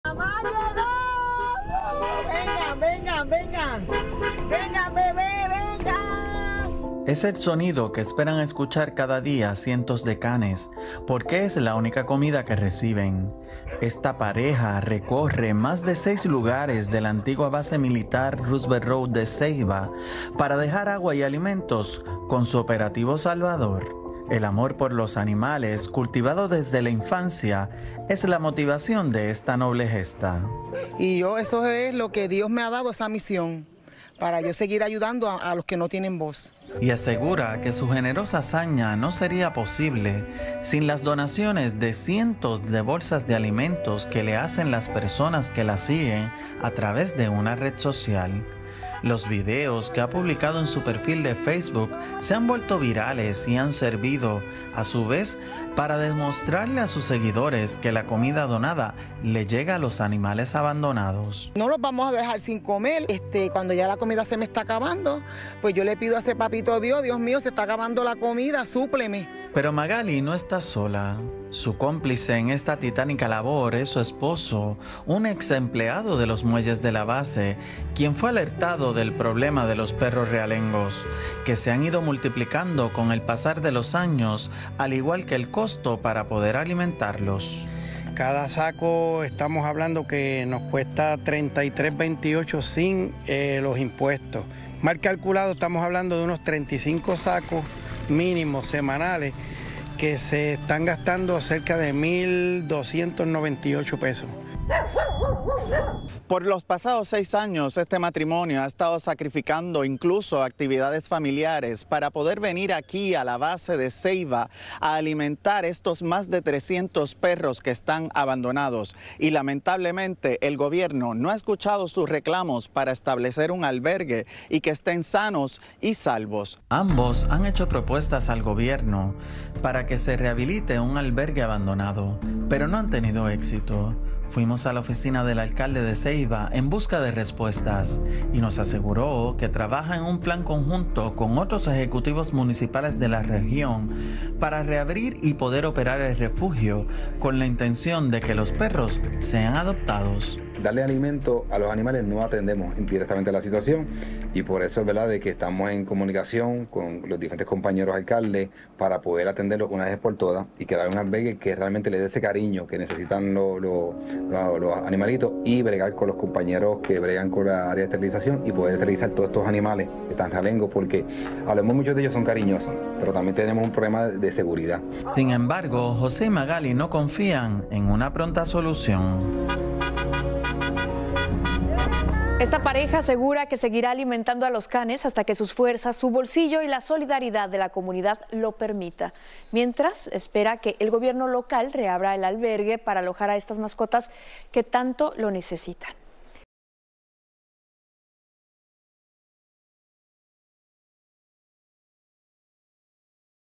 "No los vamos a dejar sin comer": pareja hispana ayuda a cientos de perros callejeros
A lo largo de 6 años, esta pareja ha dedicado su tiempo y sus recursos para alimentar a cerca de 300 perritos callejeros en Puerto Rico.

Su amor por los animales es la motivación para cumplir con esta noble tarea. “No los vamos a dejar sin comer”, dice la pareja.